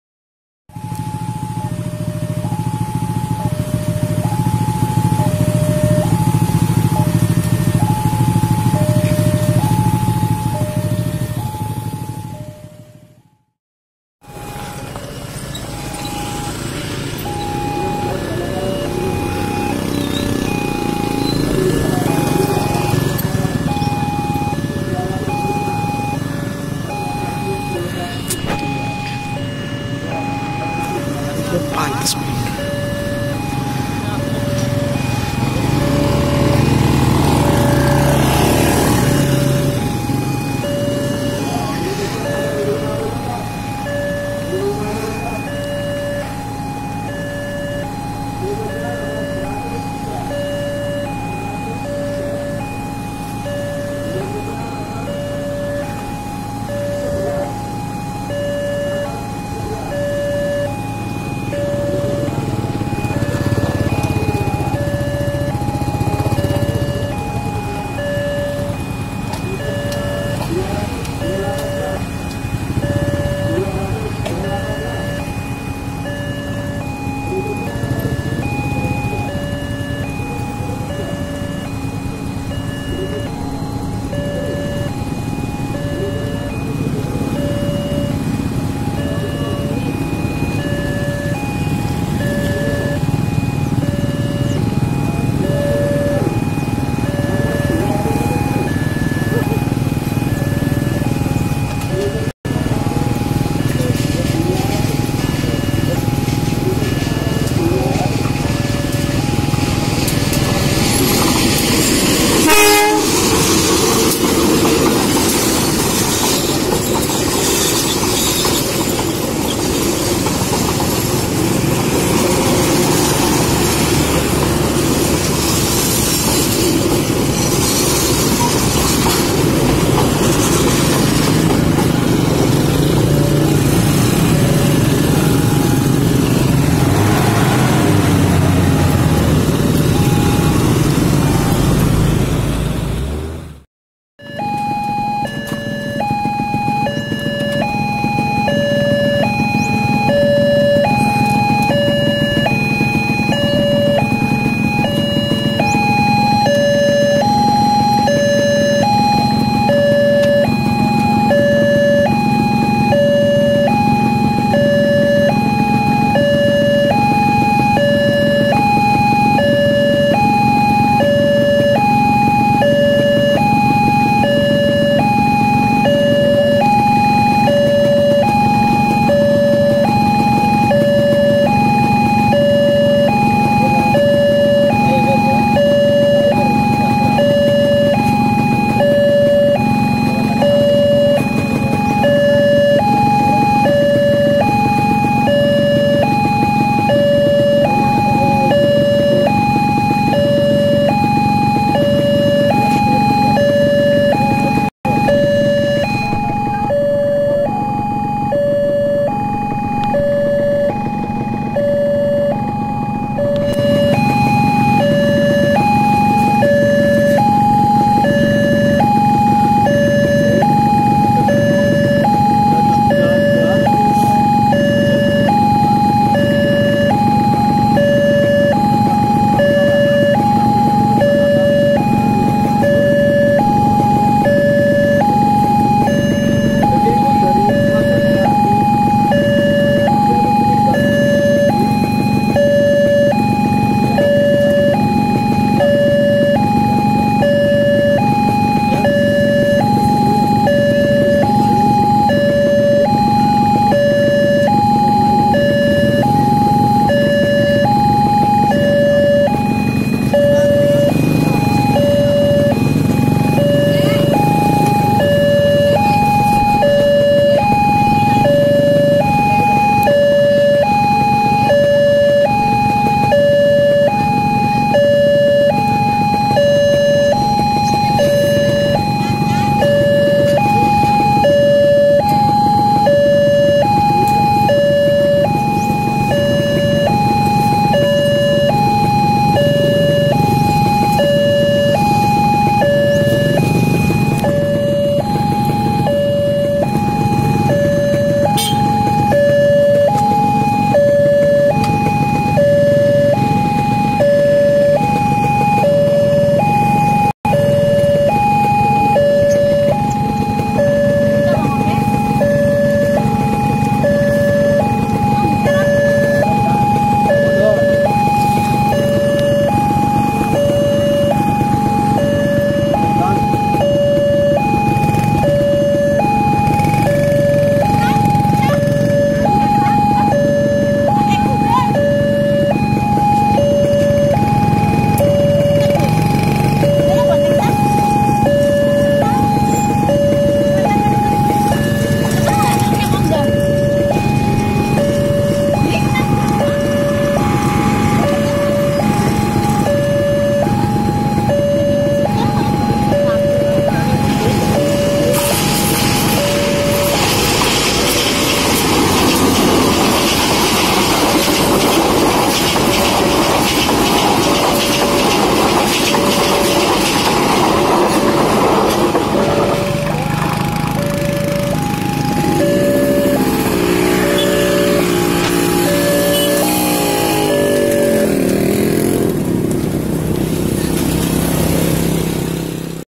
PERLINTASAN KERETA API STASIUN LEBENG JPL 461
perlintasan ini terletak di desa plajan, lebeng, Kesugihan, Cilacap Perlintasan ini letaknya sangat terpencil tidak ada angkutan kota disana kalo mo ke perlintasan ini harus jalan kaki dari jaln kemerdekaan barat ke jalan lebeng dr jalan lebeng ke perlintasan masih sangat jauh yaitu sekitar 5 KM perjalanan dengan traking kurang lebih 2 jam kalo gk sanggup ada gojek/ojek yg nemenin kalo gk punya uang y tinggal nebeng aja 😂😂😂 kaya saya. saya juga mau ngucapin selamat Idul fitri 1439 H Mohon maaf lahir dan batin. bagi yg belum subscribe silakan subscribe supaya nisa mengetahui JPL2 yg ada di Daop 5
alarm ini adalah bekas alarm JPL 468 Maos Februari 2018 :